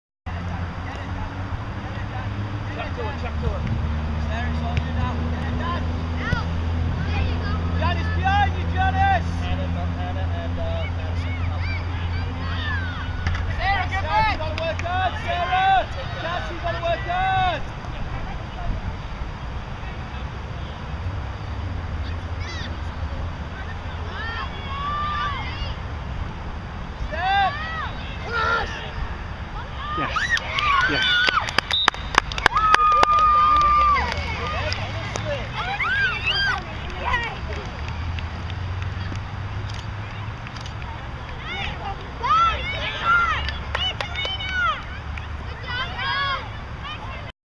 SJEB Wombat Warriors vs NJ Crush
Angelina drop passes and Devin shoots off the defender and shoots again for the goal!